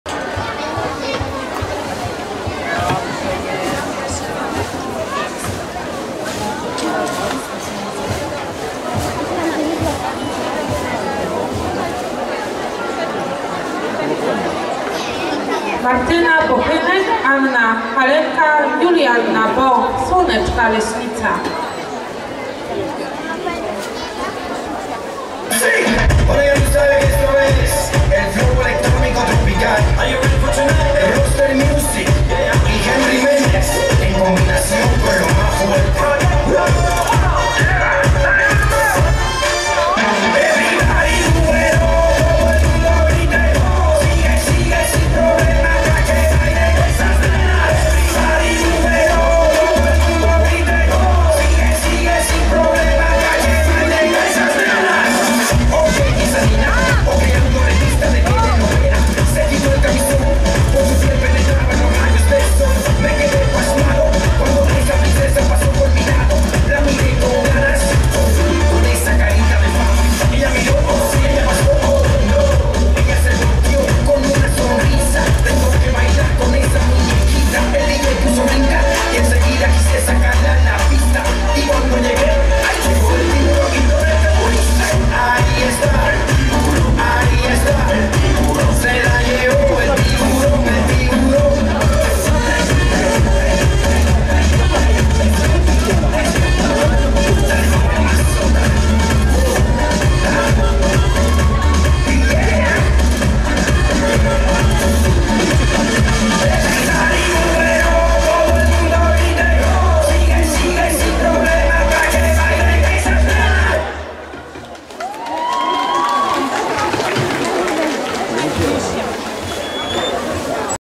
Martyna Bochynek, Anna Halenka i Julianna Bąk MP 2017
Mażoretki Słoneczka (Leśnica, Polska), 7 miejsce w kategorii pom-pom duo/trio kadetki, XIX Mistrzostwa Polski Mażoretek, Kędzierzyn-Koźle, 20 maja 2017 r.